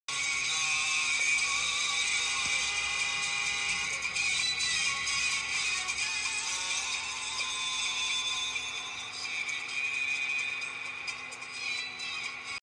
Kahramanmaraş’ta yemek firmasının aracı trafiği tehlikeye düşürdü!
Kahramanmaraş’ın Onikişubat ilçesinde ters yöne giren bir yemek firmasına ait olan araç trafiği tehlikeye düşürdü.

Kahramanmaraş Manşet Gazetesi